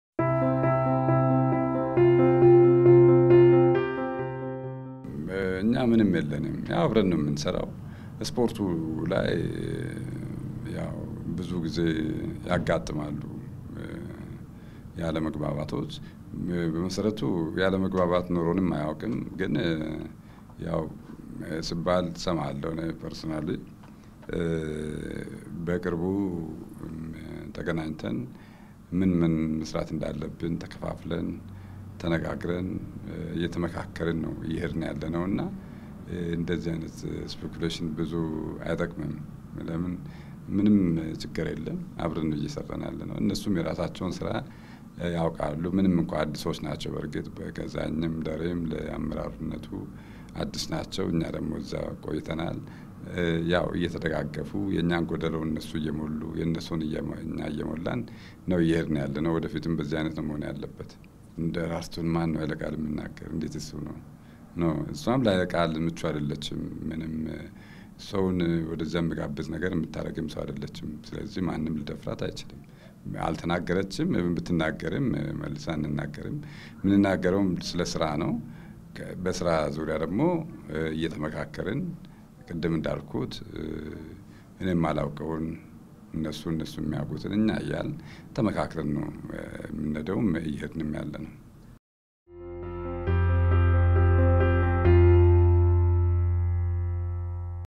“ከአትሌቲክስ እና የኦሎምፒክ ኮሚቴው አመራሮች በመነጋጋር ነው እየሰራን ያለነው”- ዶ/ር አሸብር ወ/ጊዮርጊስ
ከአሁን ቀደም በኢትዮጵያ አትሌቲክስ ፌዴሬሽን እና በኢትዮጵያ ኦሎምፒክ ኮሚቴ ከፍተኛ አመራሮች መካከል ከተፈጠረው እሰጣ-ገባ ጋር በተያያዘ አል ዐይን አማርኛ የጠየቃቸው የኢትዮጵያ ኦሎምፒክ ኮሚቴ ፕሬዝዳንት ዶ/ር አሸብር ወ/ጊዮርጊስ “በመነጋጋር ነው እየሰራን ያለነው” ብለዋል፡፡